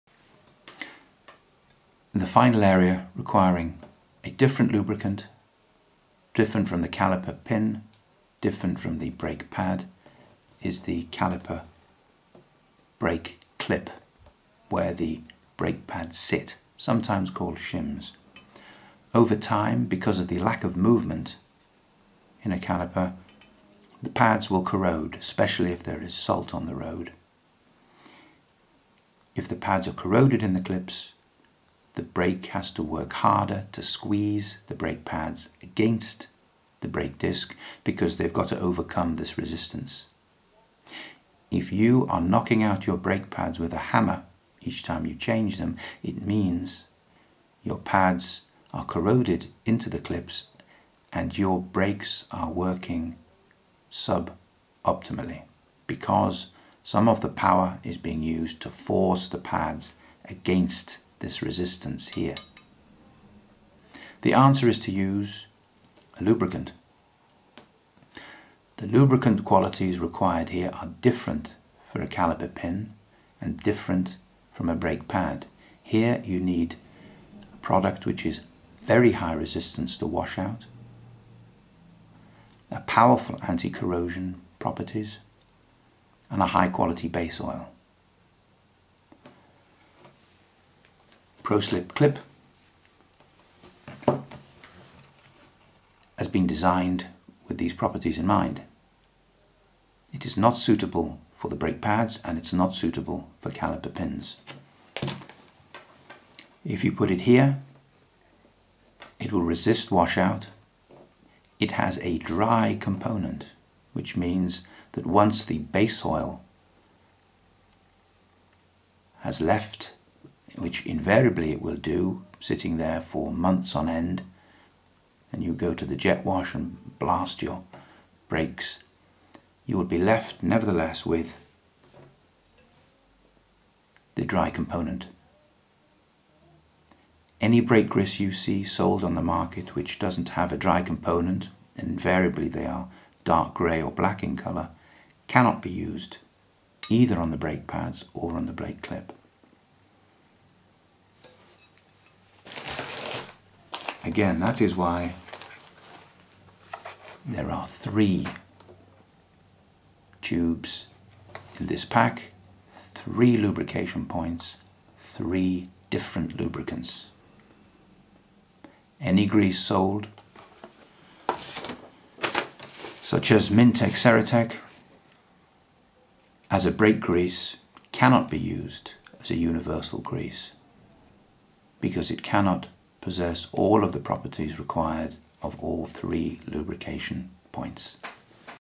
ProSlip vs Ceratec
ProSlip has three tubes in the pack - there are three areas on a caliper which need lubrication and the lubrication requirements of each area is different - ProSlip CLIP is a dry low friction lubricant with a powerful anti-corrosion package in a high quality base oil - Made by Shell